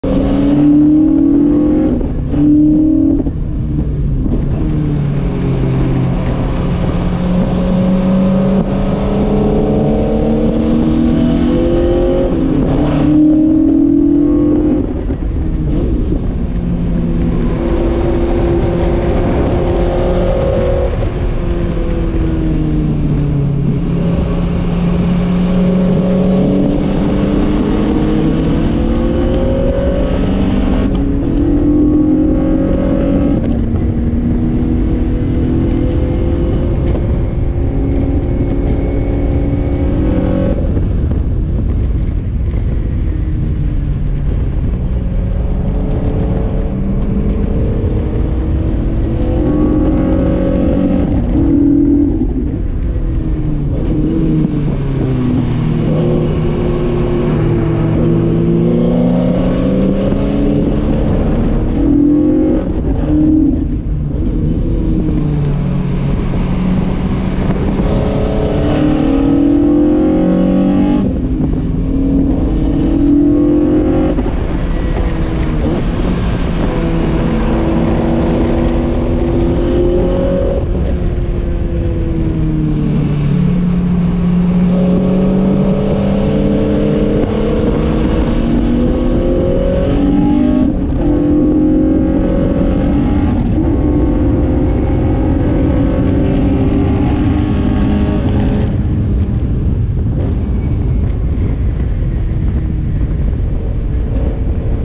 Ginetta G16 and porsche 917 driven by Mike Wilds at brands hatch
testing day with Ginetta G16  (dare)
2.3 cosworth power , 590kg